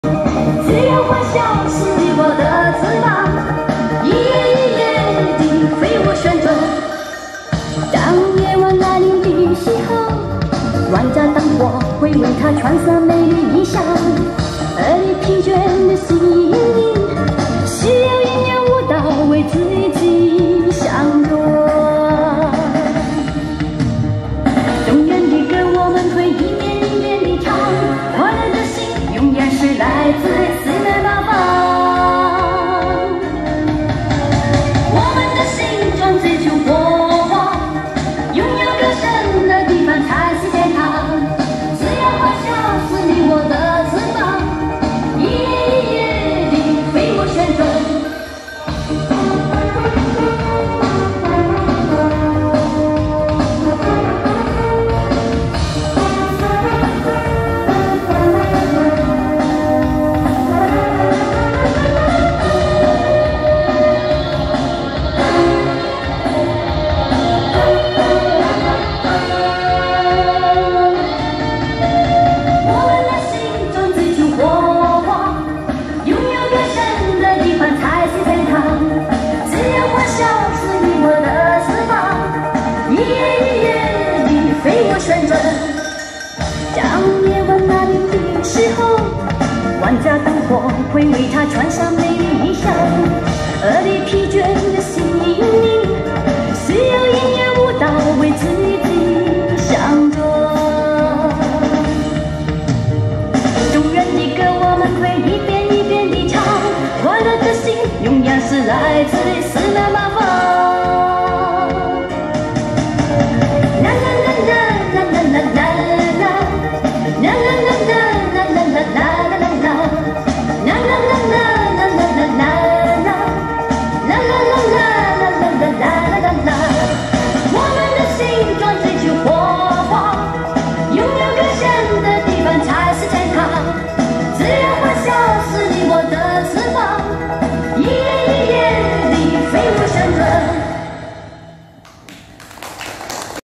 飛舞時刻，高紅玫，疫境中，戴上口罩，樂意為歌迷獻唱演出。
大家時時刻刻，也飛舞時刻。